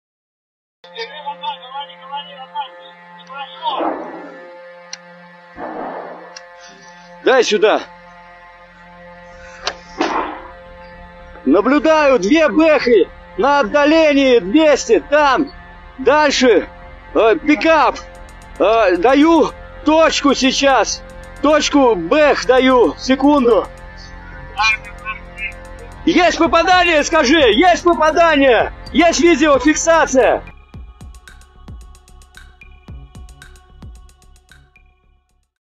A Ukrainian BMP-2 and what appears to be a M80A infantry fighting vehicle were destroyed
A Ukrainian BMP-2 and what appears to be a M80A infantry fighting vehicle were destroyed by the Russian Wagner PMC near Zaliznyanske, Donetsk Oblast, Ukraine Russia Military Video, GoPro Cam Footage